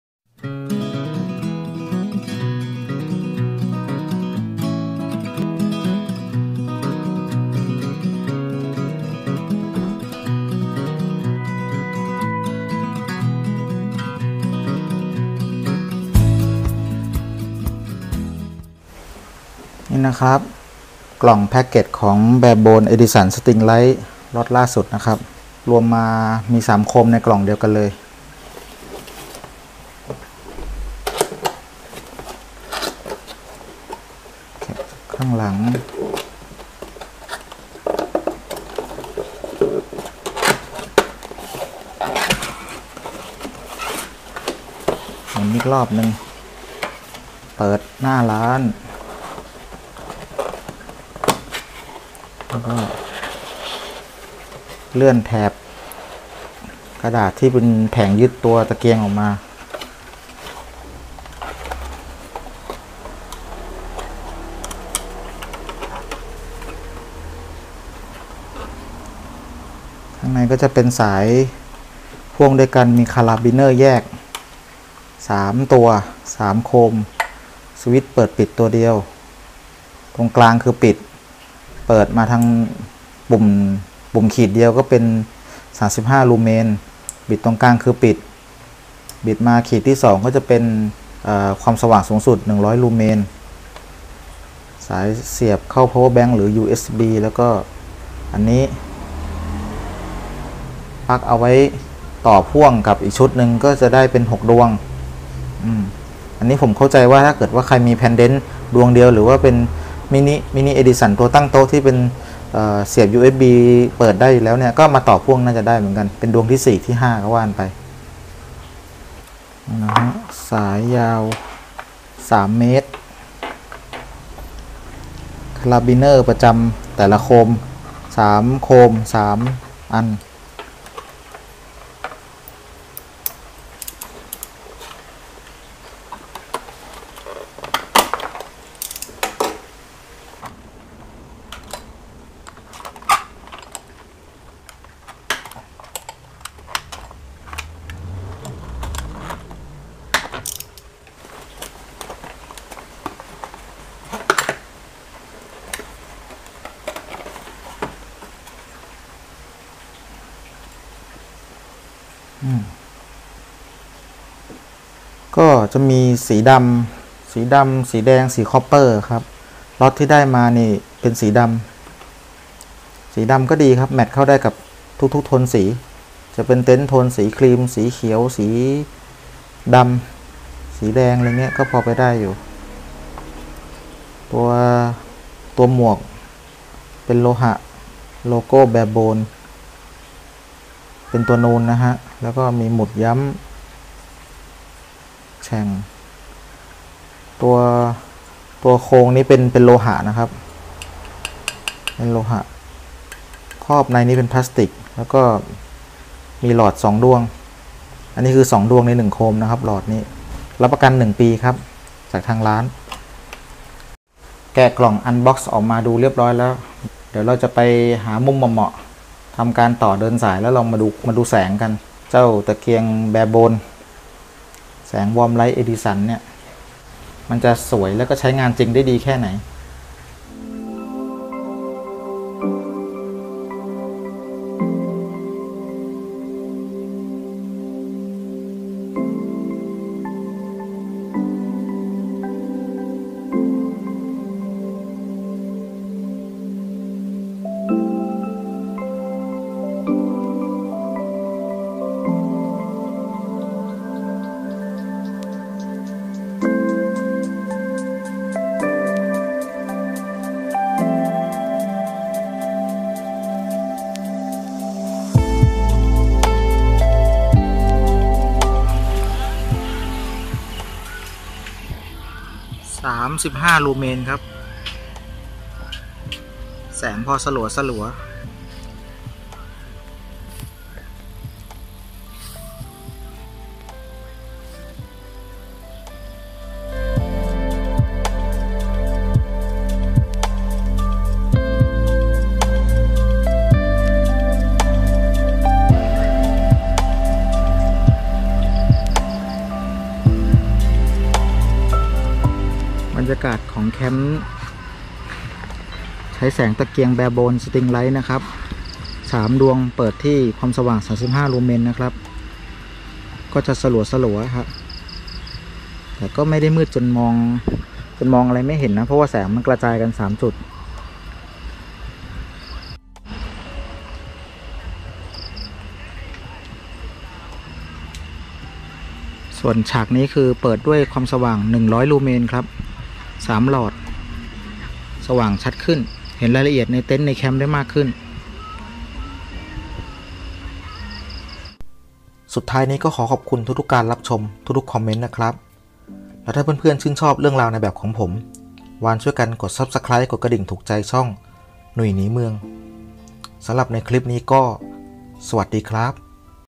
รีวิว Bearbones Edison string light
เป็นทั้งตะเกียง+ ไฟราวตบแต่งสร้างบรรยากาศ+ รูปทรงวินเทจถ่ายรูปสวย วัสดุดีงาม
ทนทาน ไม่จุกจิก เสียบไฟผ่านusb และต่อพ่วงได้มากถึง6ดวง